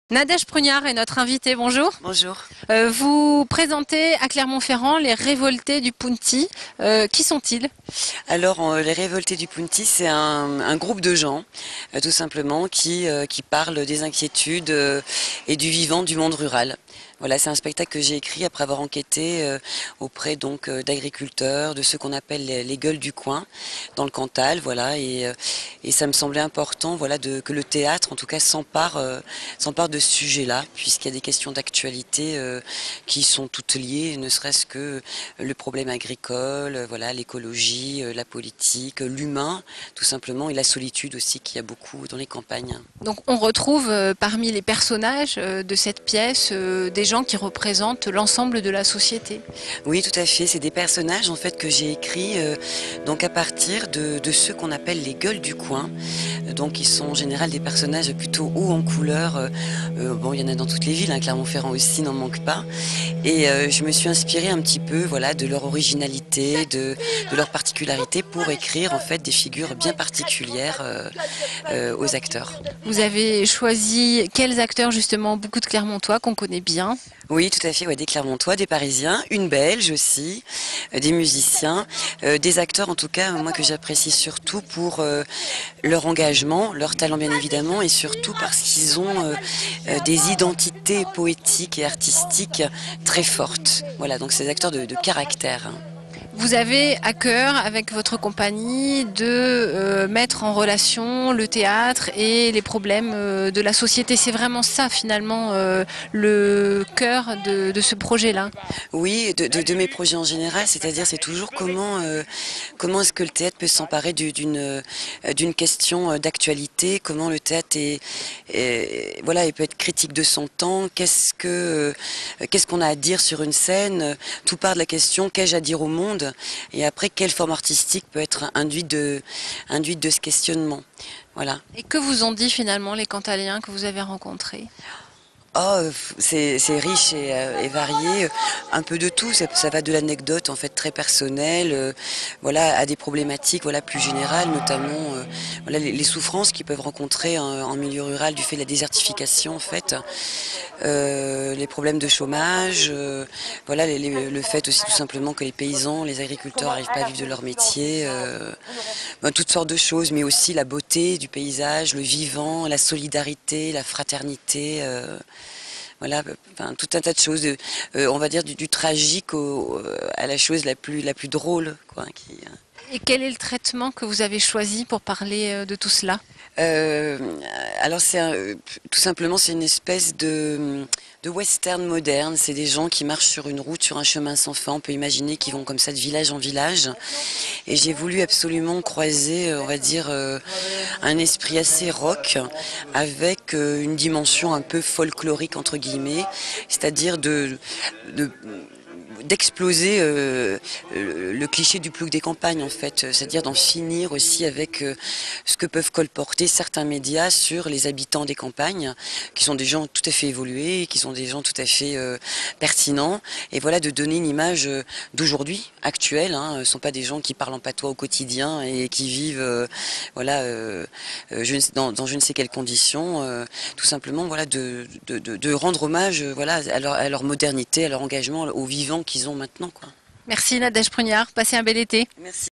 Les révoltés du Pounti de Nadège Prugnard
Sites webs/blogs, copiez cette vidéo avec le bouton "Intégrer" !
 Rencontre avec l'auteur et metteur en scène, Nadège Prugnard, qui nous fait découvrir sa nouvelle création, « Les révoltés du Pounti ».